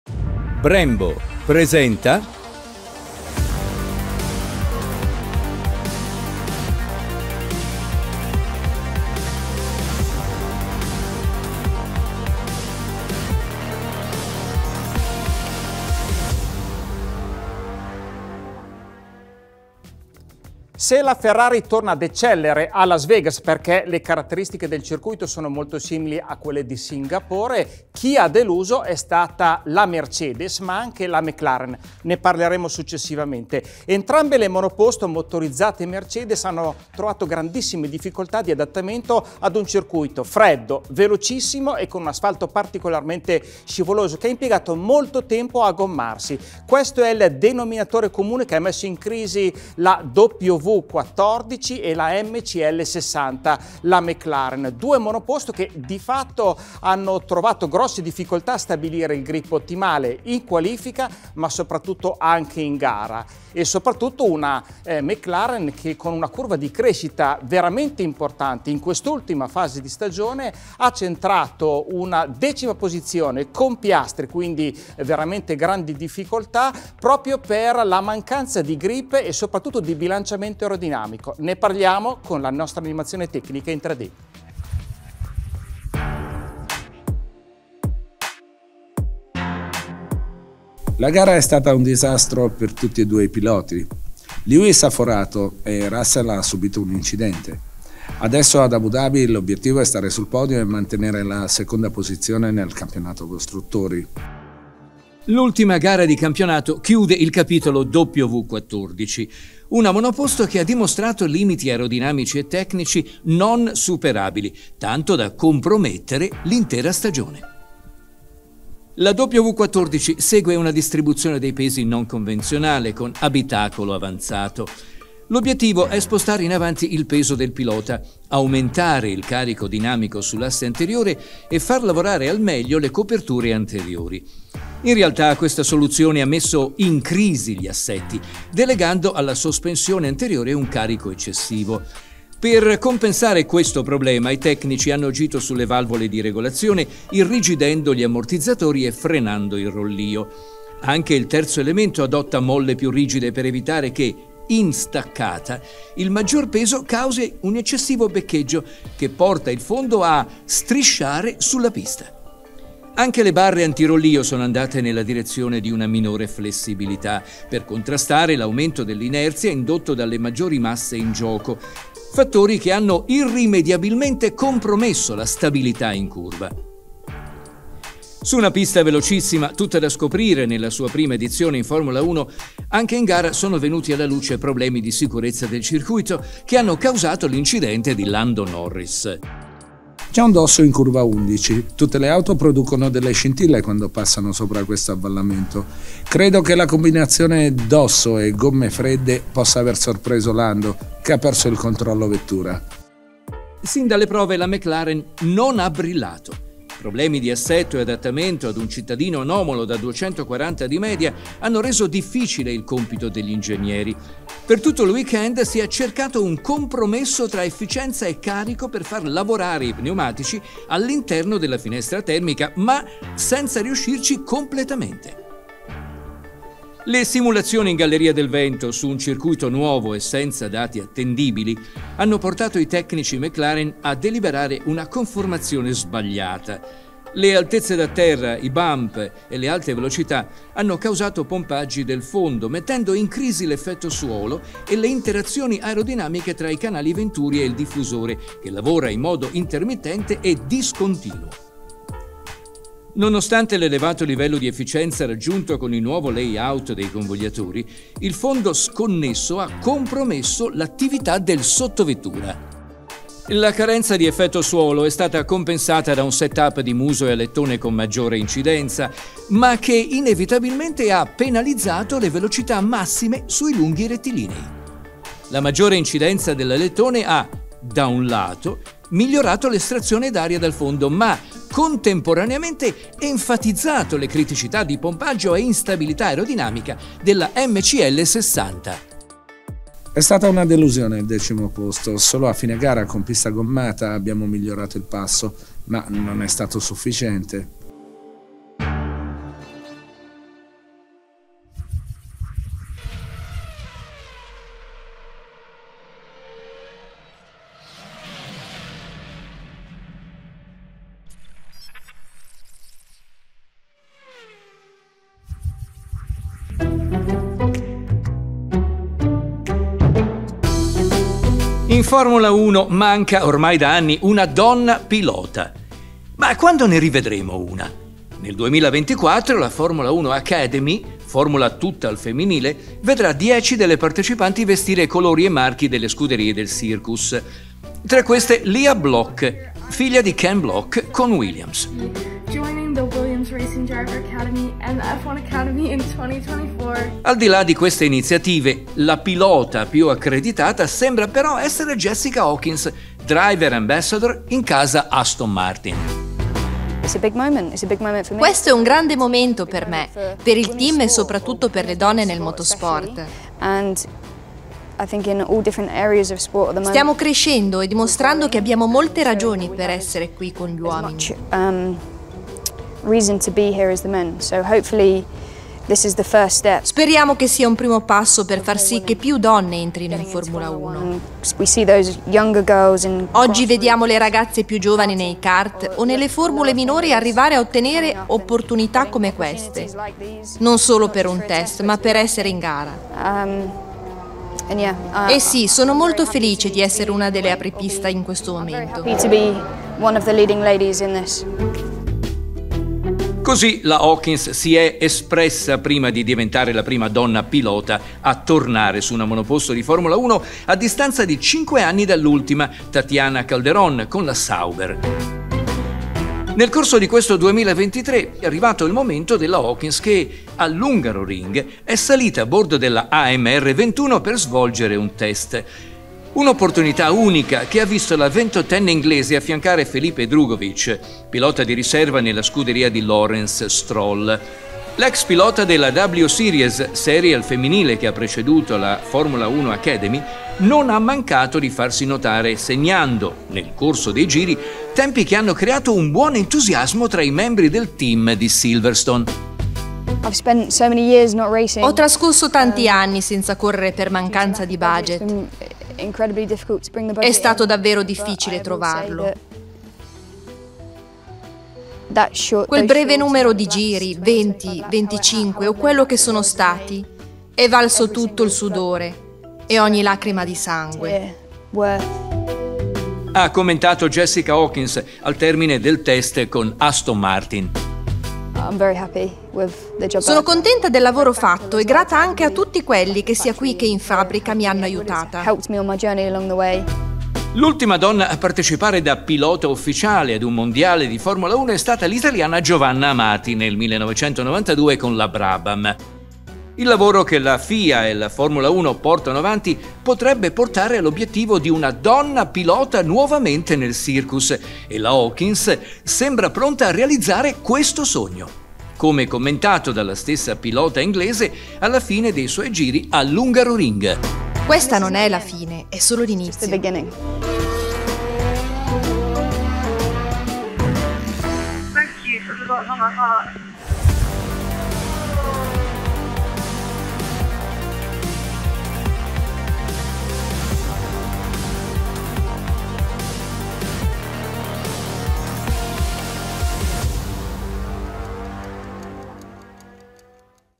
ChronoGP-S05:39 Parte 2 di 3 - Mercedes e McLaren male a Las Vegas | Donne in F1
Would you like to watch our videos with subtitles in your preferred language? Take a look at this tutorial, it's very easy.

L’ultima gara di campionato chiude il capitolo W14, una monoposto che ha dimostrato limiti aerodinamici e tecnici non superabili tanto da compromettere l’intera stagione.

In F1 manca, ormai da anni, una donna pilota: ma quando ne rivedremo una? Nel 2024 la F1 Academy, formula tutta al femminile, vedrà 10 delle partecipanti vestire colori e marchi delle scuderie del Circus.

00:00 - Mercedes e McLaren
05:16 - Donne in F1

Grafiche e 3D a cura di Alta Vision Network s.r.l.